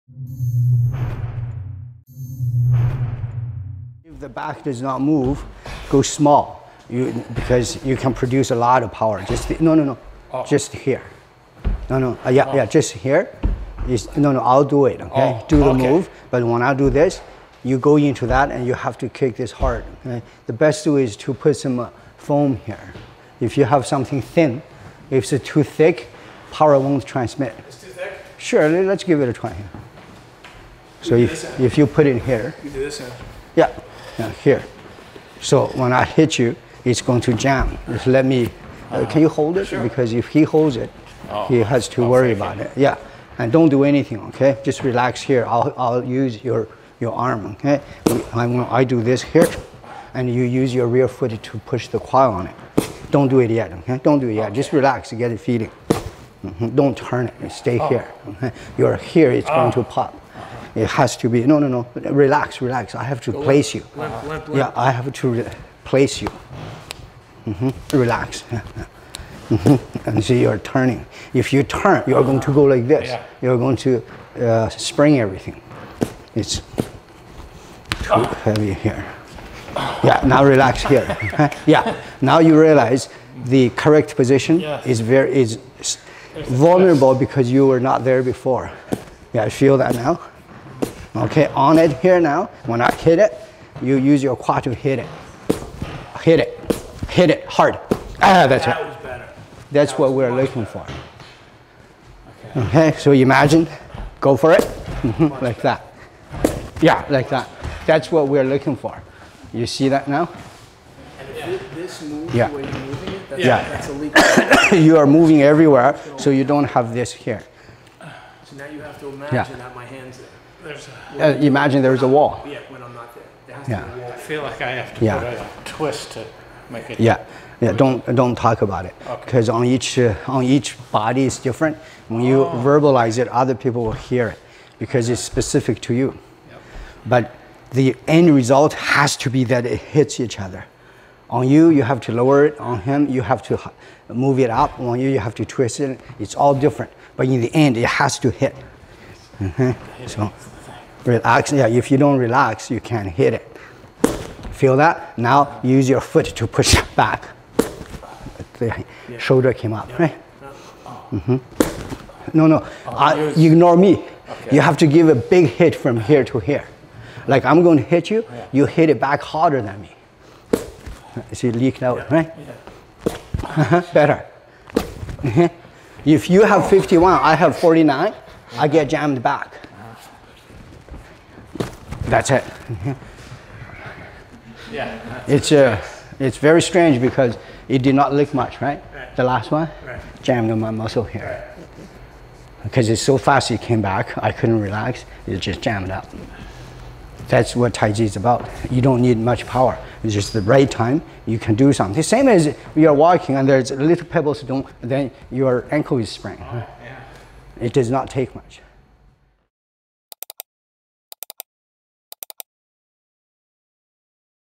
Chen Zhonghua "It Does Not Take Much" Practical Method Workshop Wittmann, AZ 2023
Master Chen Zhonghua demonstrates the concept of "It Does Not Take Much." During the Arizona Push Hands Workshop in Wittmann, AZ in July of 2023.